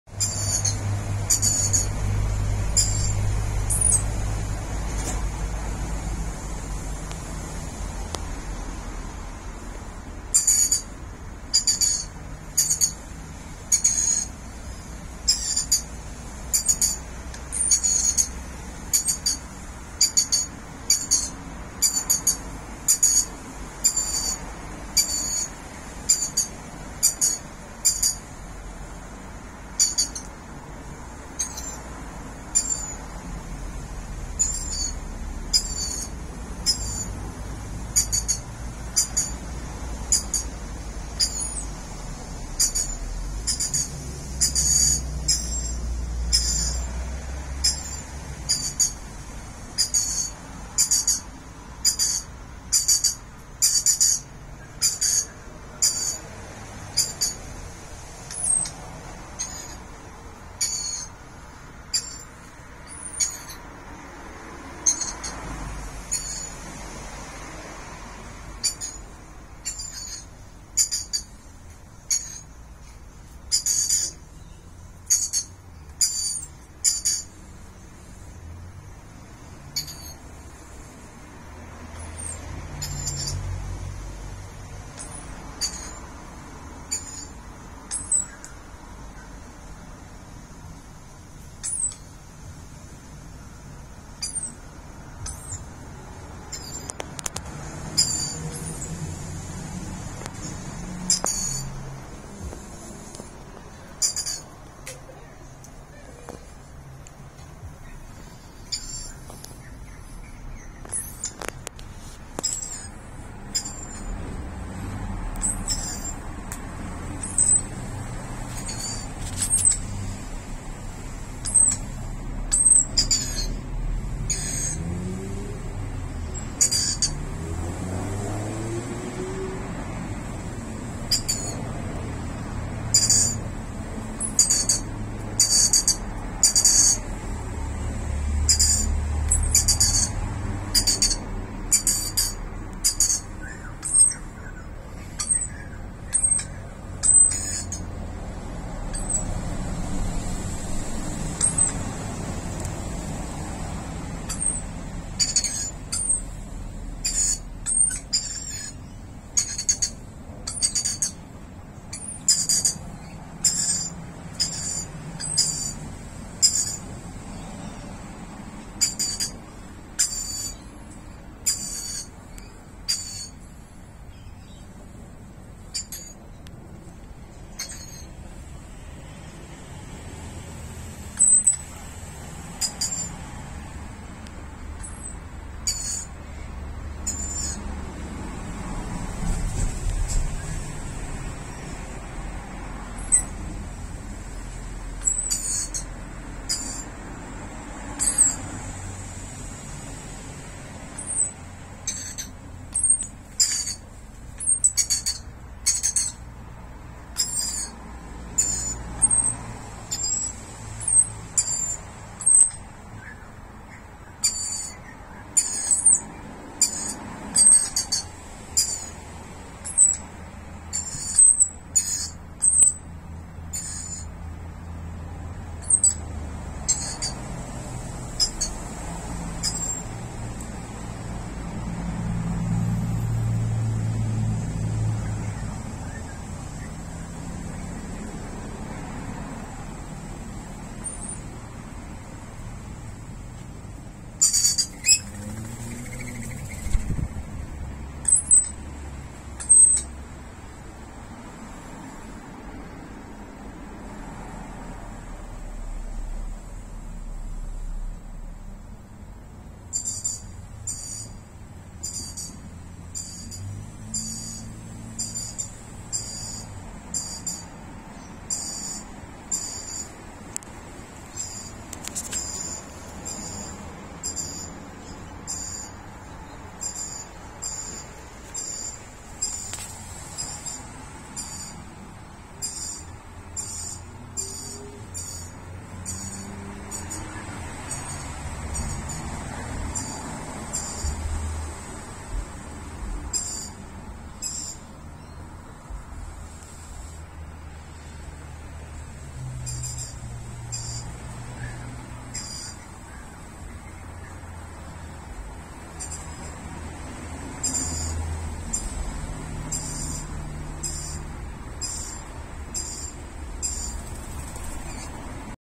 三隻彩虹吸蜜鸚鵡吃麵包，吃完往我的方向飛 11(Trichoglossus haematodus)
三隻彩虹吸蜜鸚鵡聚在地上吃麵包，完全不在意我拍攝，吃完還往往我的方向飛